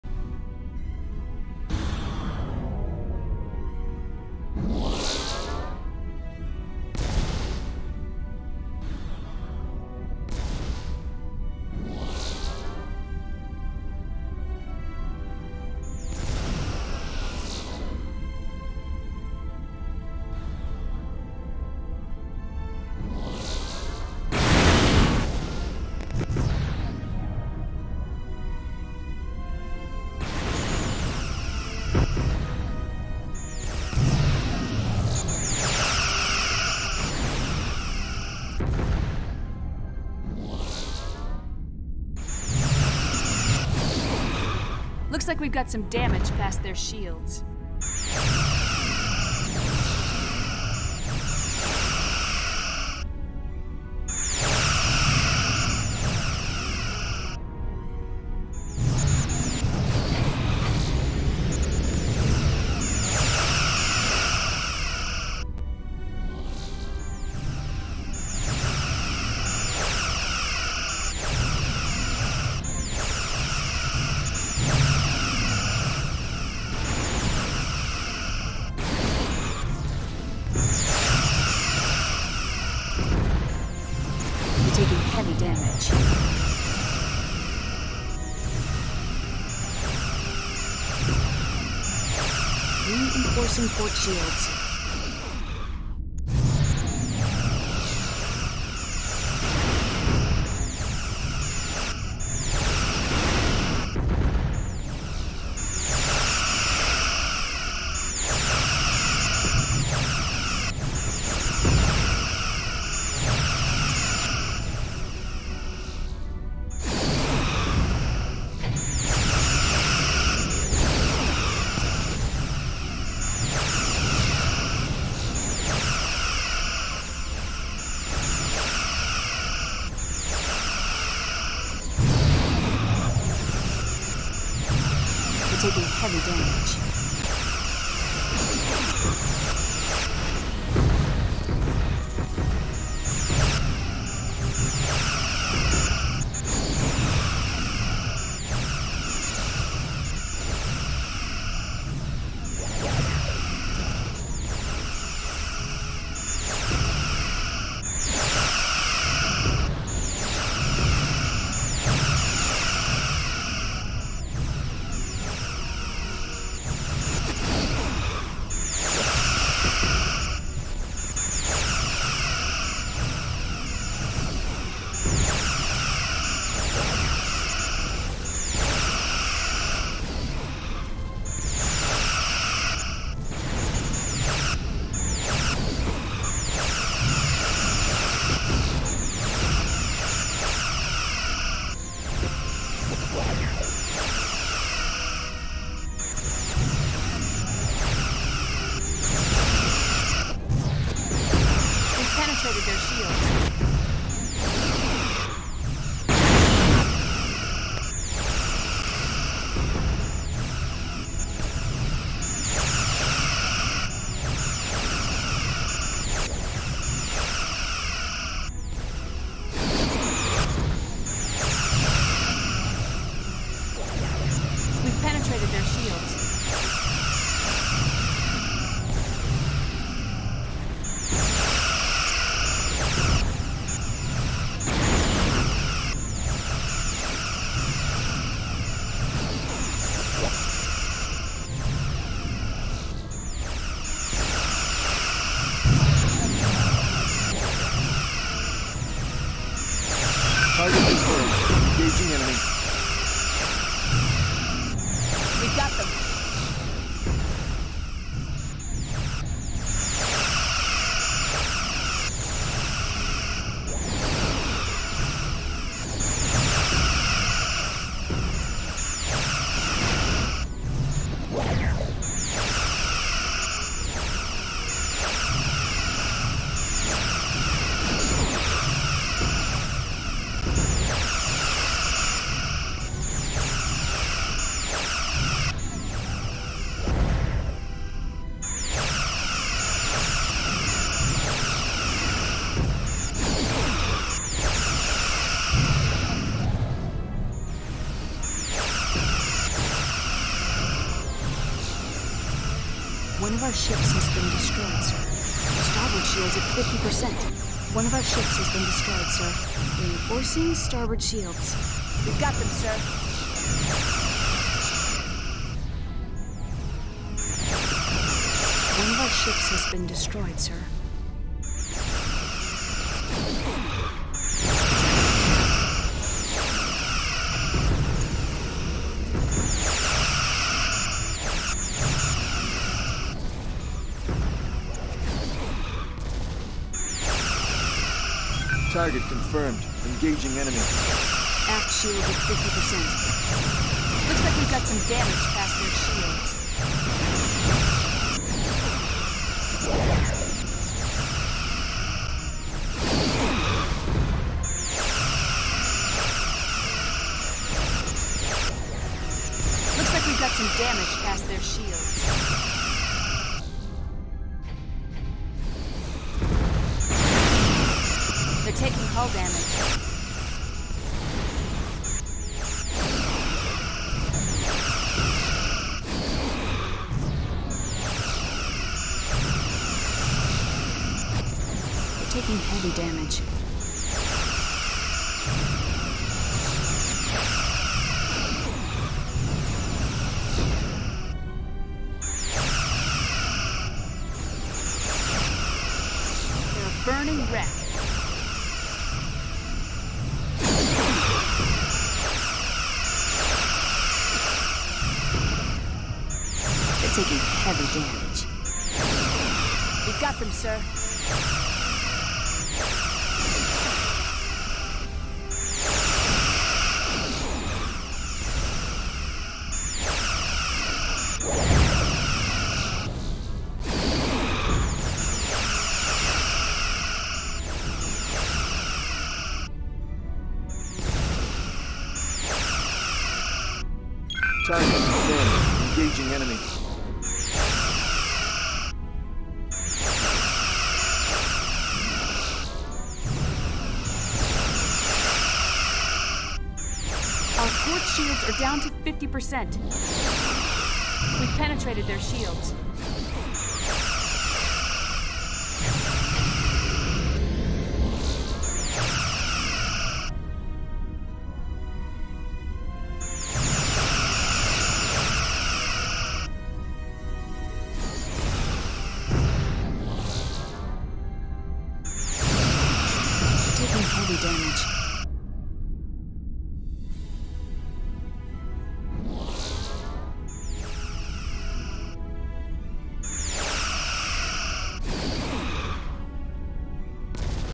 Earth Space Dock Goliath
Earth Space Dock defends Earth against an assimilated federation task force.